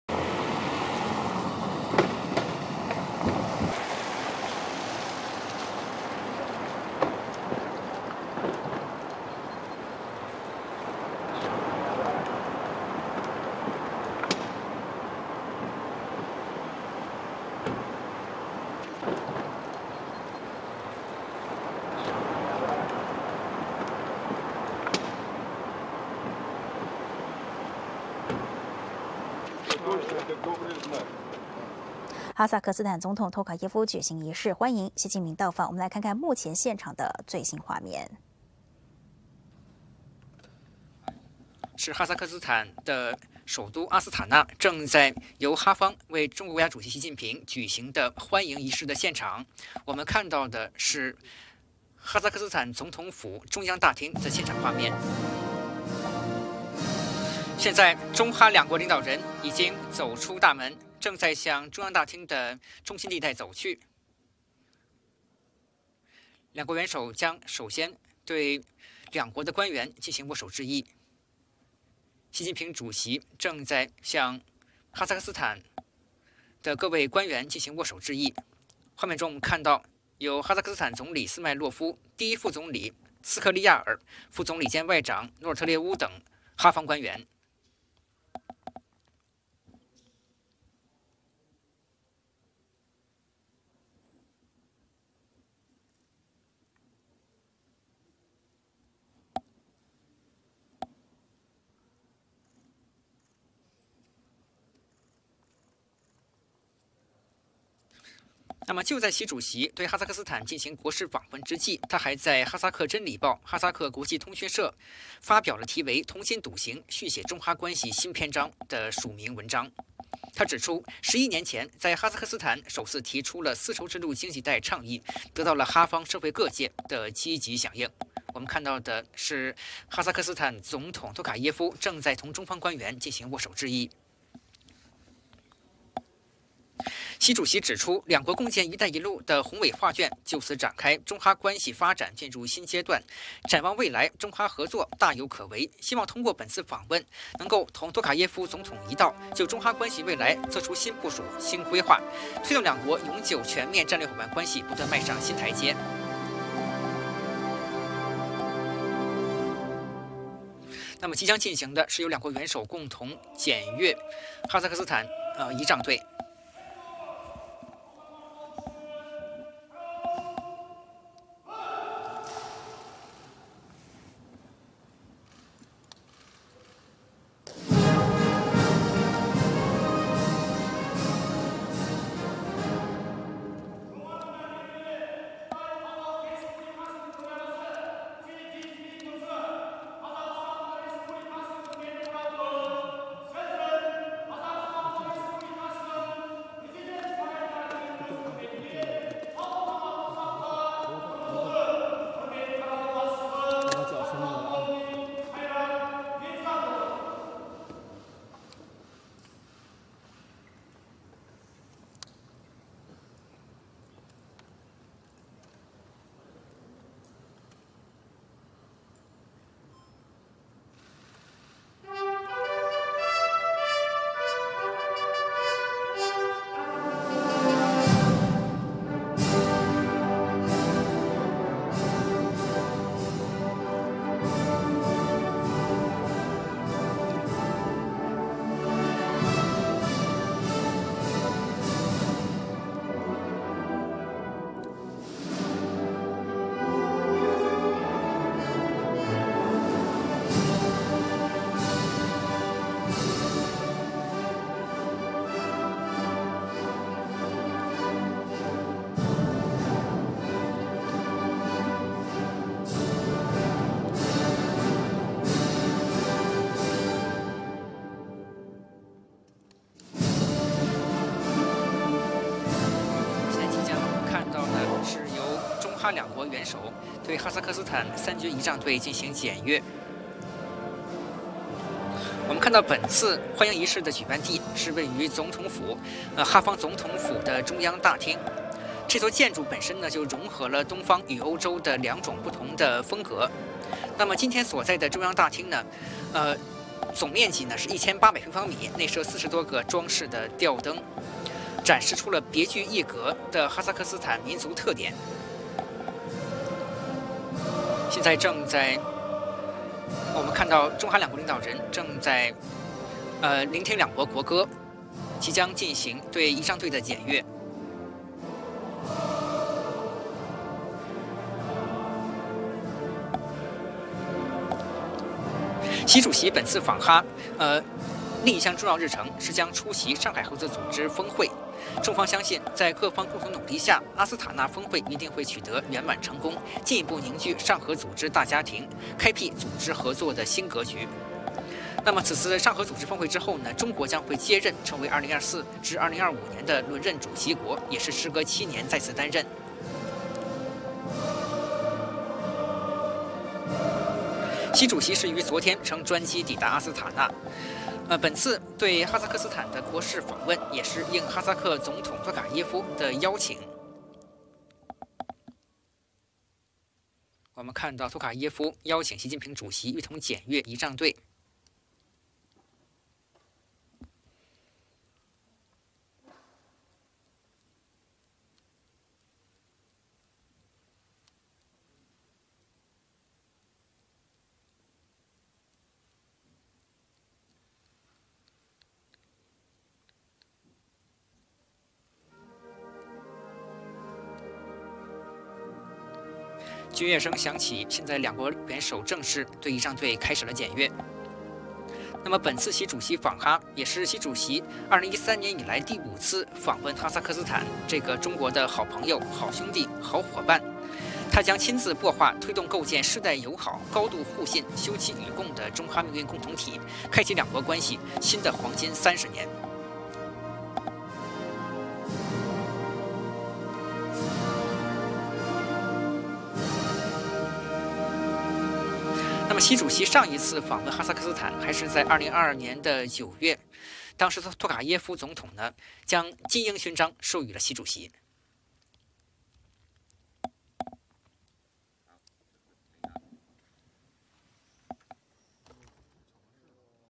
【直播回放】習近平訪問哈薩克斯坦 哈方舉行歡迎儀式｜鳳凰衛視
當地時間7月3日，正在對哈薩克斯坦進行國事訪問的國家主席習近平出席哈總統托卡耶夫在總統府舉行的歡迎儀式。習近平主席抵達時，哈方多架戰機在總統府上空拉出中國國旗色彩煙歡迎。這是哈方首次這樣安排。

*嘉賓言論不代表鳳凰衛視立場
【鳳凰衛視官方Facebook】